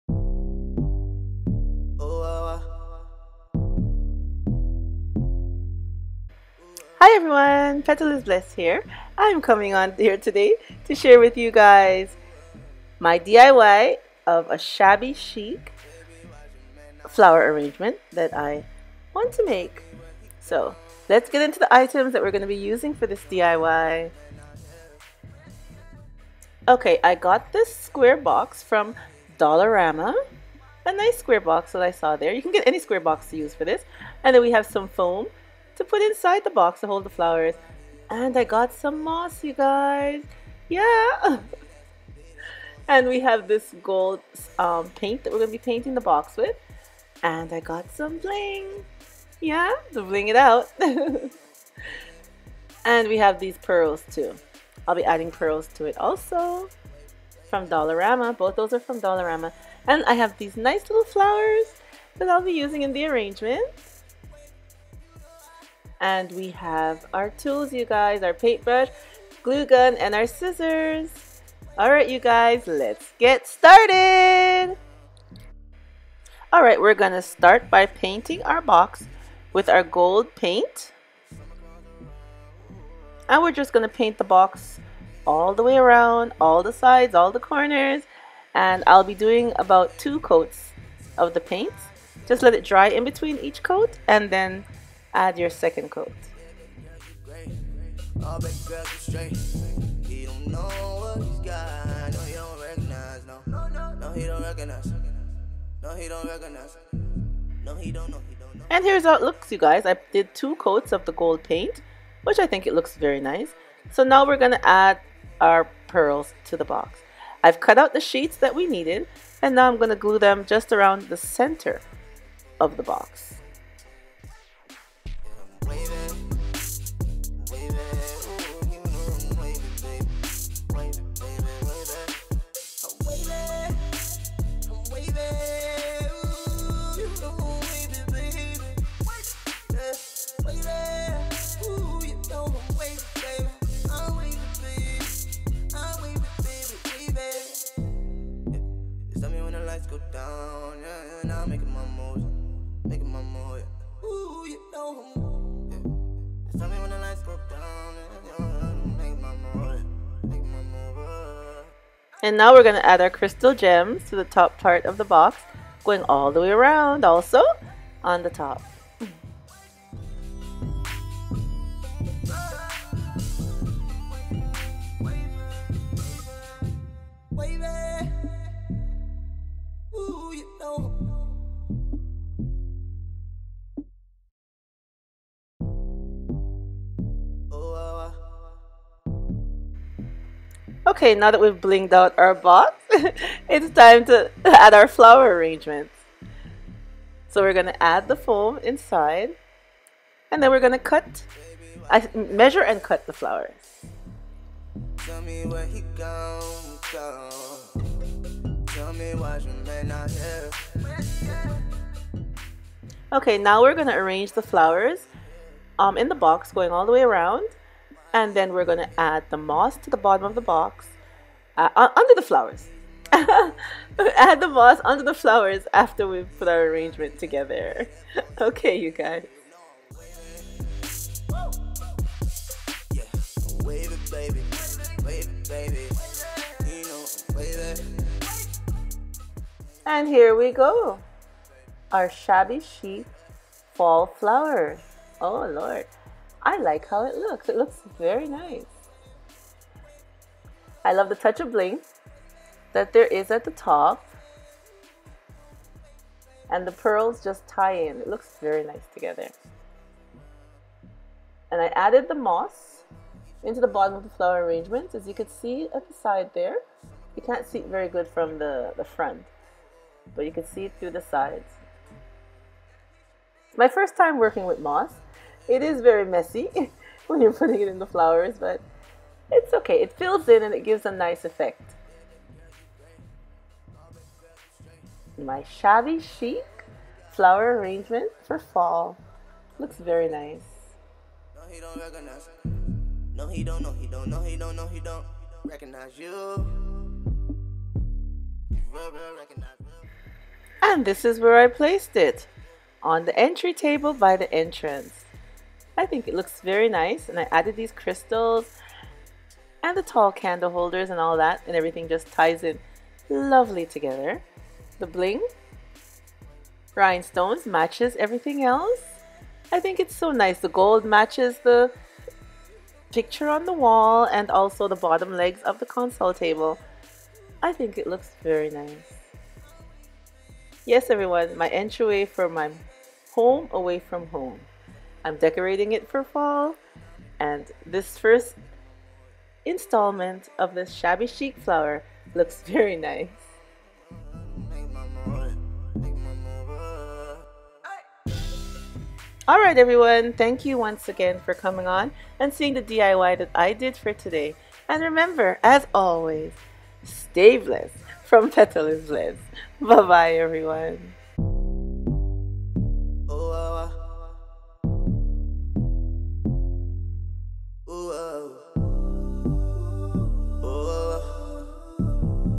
DIY DOLLAR STORE SHABBY CHIC FALL HOME DECOR | 🌹 SHABBY CHIC DECOR IDEAS🌹 | SHABBY CHIC FLOWERS
Hi Everyone, I'm back with a gorgeous Shabby Chic DIY Fall Home Decor. This Shabby Chic Floral Arrangement you can make for under $12! Make this Shabby Chic Decor in just a few easy steps for fabulous Shabby Chic decorating! Have fun!